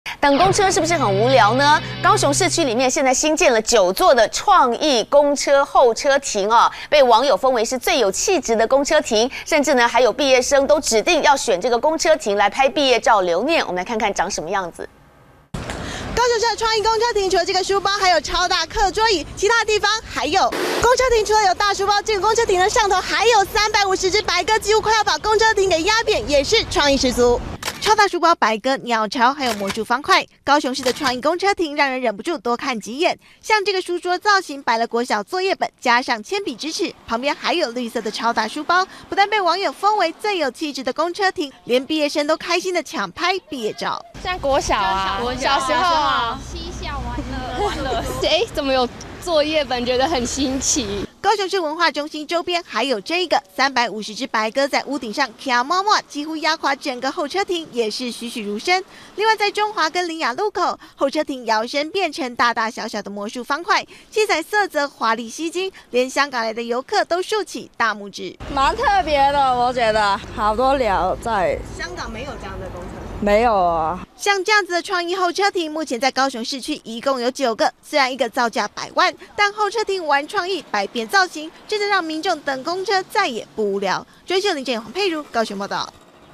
【中天】6/06 等公車不無聊！高市候車亭大書包創意吸睛
在高雄市等公車，現在不無聊囉！因為市區內，新建了九座創意公車候車亭，其中有個書桌造型，加上超大書包，作業本還有文具用品，更是被網友封為「最有氣質的候車亭」！此外，藝術家還把350隻白鴿，通通堆在公車亭屋頂上，還有七彩鮮豔的超大魔術方塊，都超吸睛，連畢業生都指定選公車亭拍畢業照留念。